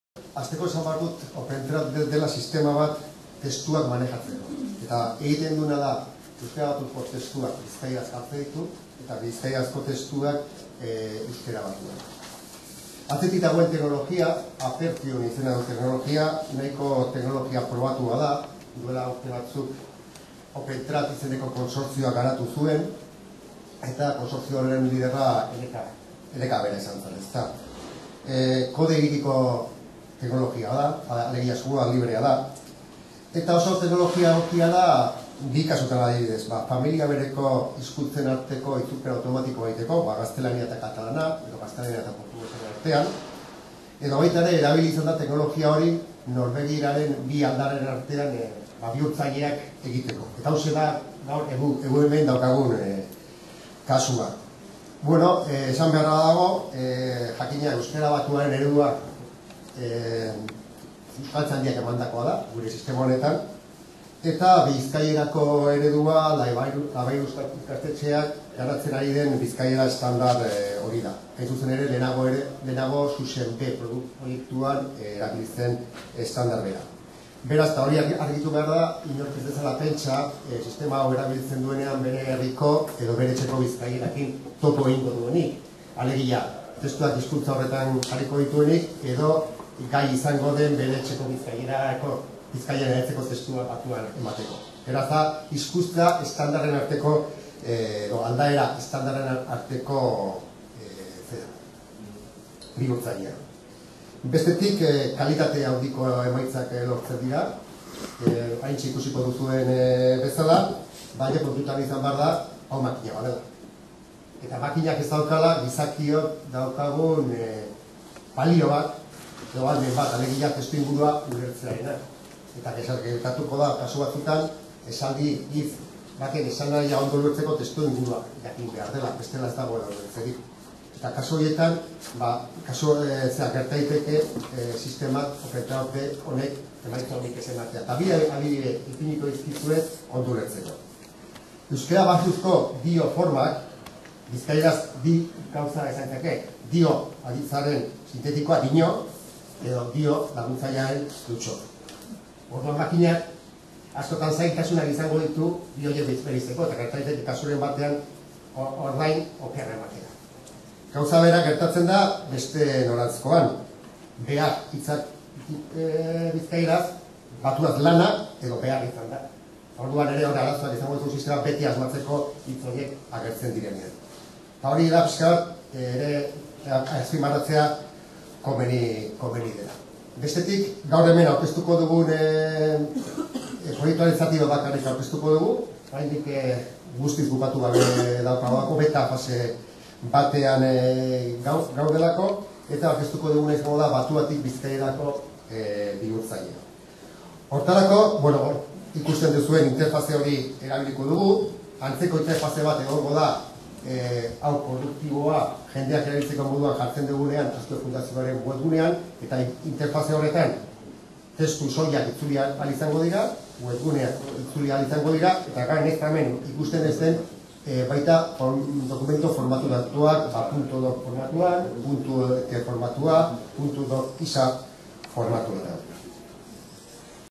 Batua-bizkaiera bihurtzailearen aurkezpena 2
Josune Ariztondok (Azkue Fundazioa) eta Iñaki Irazabalbeitiak (Eleka) "Batua-bizkaiera bihurtzailea" aurkezten dabe.